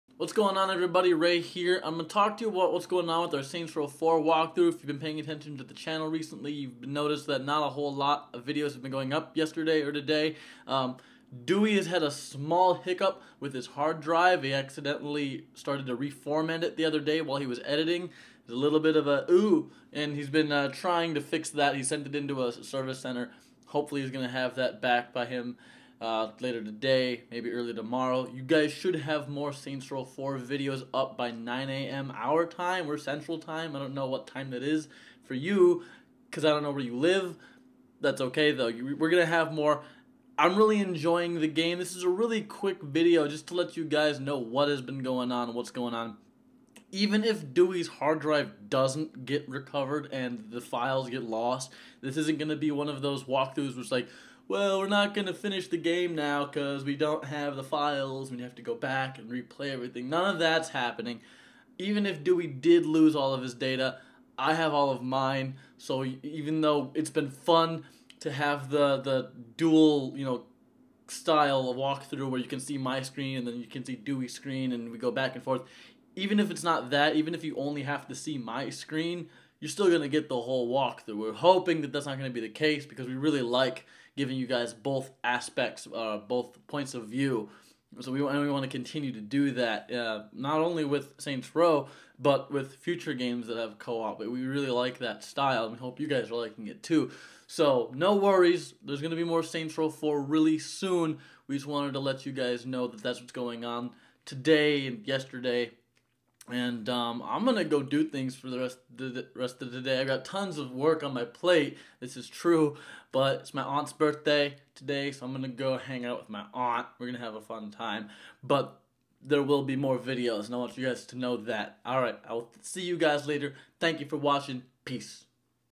Saints Row 4 Vlog Update with Ray!
Ray is here to give you a quick update on our Saints Row 4 wakthrough. No worries, all is going well. In fact, Ray is super pumped to play even more Saints Row 4 in the very ner future.